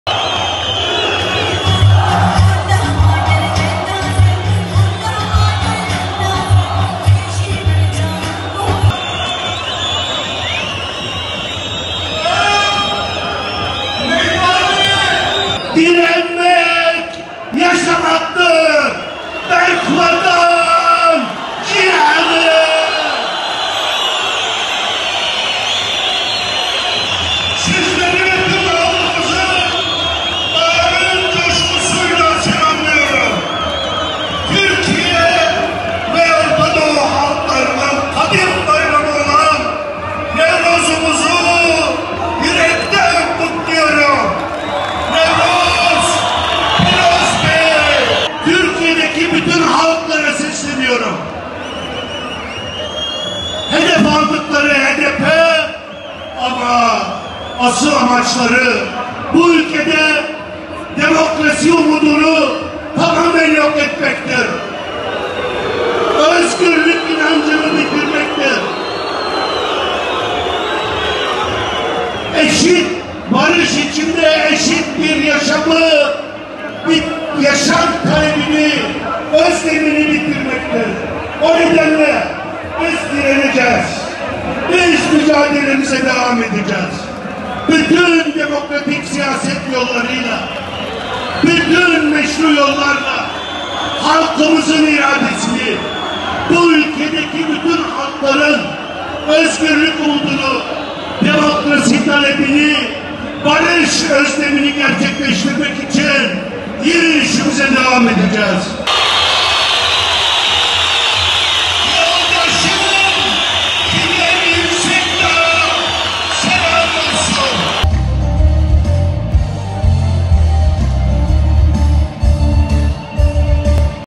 Diyarbakır’da Nevroz kutlaması - Mithat Sancar: "Meydanlardaki milyonları nereye kapatacaksınız?"
Diyarbakır’da, geçen yıl koronavirüs salgını nedeniyle yapılamayan Nevruz kutlaması, bu yıl büyük bir kalabalıkla kutlandı. Yoğun güvenlik önlemleri alınan kutlama alanına vatandaşlar HES kodu kontrolüyle alındı. Halkların Demokratik Partisi milletvekilleri, Başak Demirtaş ve sivil toplum kuruluşu temsilcilerinin de katıldığı kutlamada konuşan Halkların Demokratik Partisi Eş Genel Başkanı Mithat Sancar, “Hadi HDP’yi kapattınız, meydanlardaki bu milyonları nereye kapatacaksınız? HDP dükkan mı ki kapatıyorsunuz? Başaramayacaksınız, demokratik siyasette ısrarımızı sürdüreceğiz” dedi.

Haber: Ferit Aslan
Kurgu: Özgün Özgül

Teşekkürler!